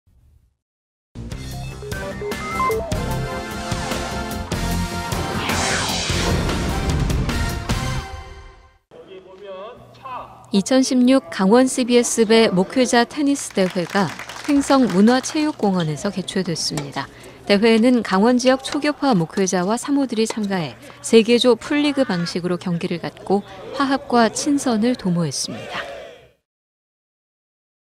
161101강원cbs배목회자테니스대회
2016 강원CBS배 목회자 테니스대회가
횡성문화체육공원에서 개최됐습니다.
대회에는 강원지역 초교파 목회자와 사모들이 참가해
3개조 풀리그 방식으로 경기를 갖고
화합과 친선을 도모했습니다.

/16.10.31, 횡성문화체육공원